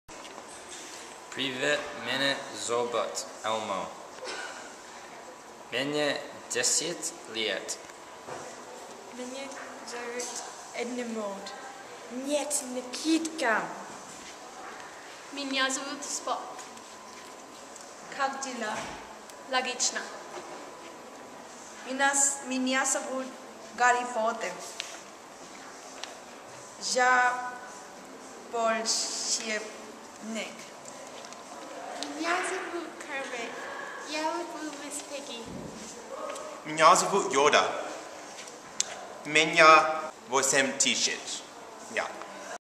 Day 1 Знакомство RB82 Beginner 2 Lesnoe Ozero
Day 1 at Lesnoe Ozero (Concordia Language Villages)
Beginner 2
Theme: Nice to meet you!